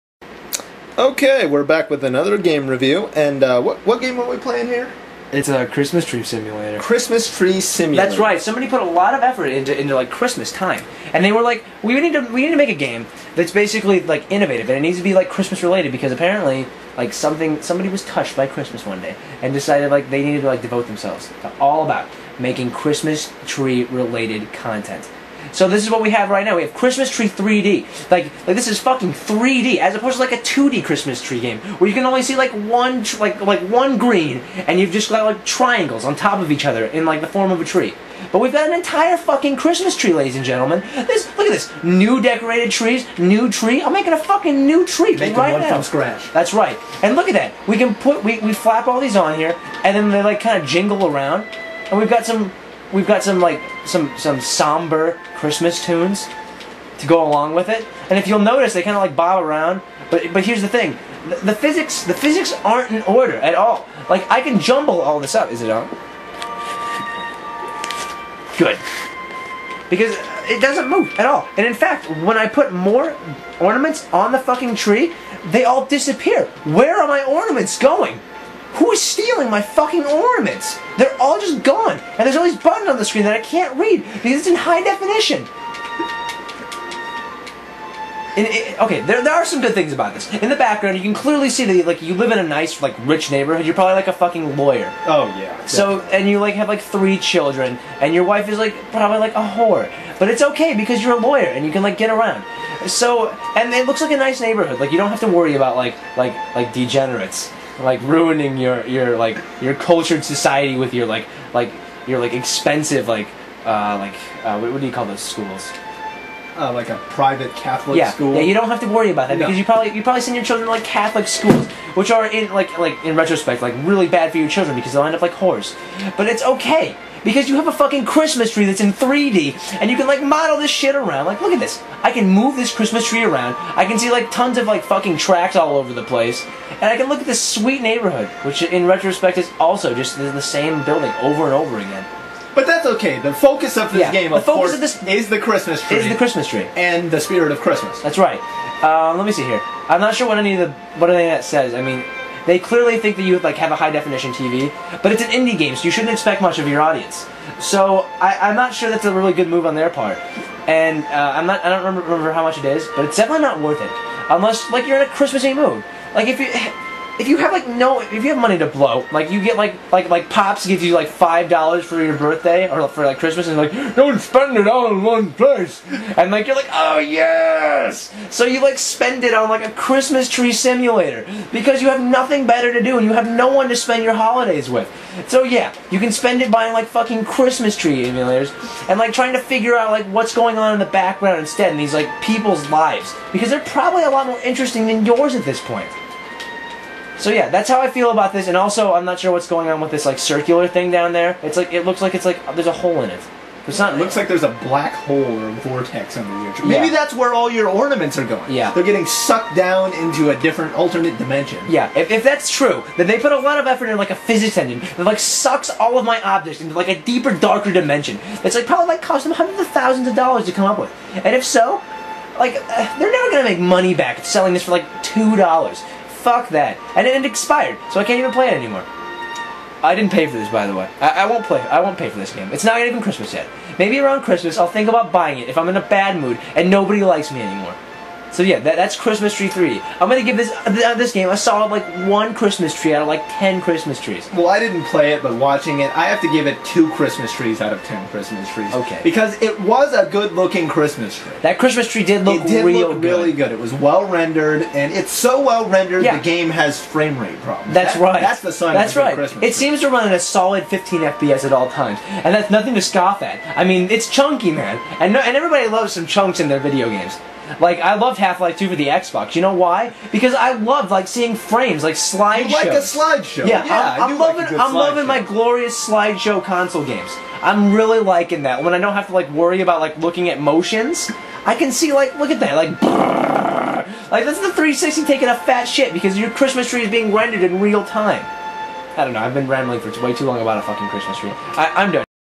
Eric & David Indie Reviews: Christmas Tree 3D (Ep. 2)
Does this virtual Christmas tree hold up to the real thing? Found out in the review.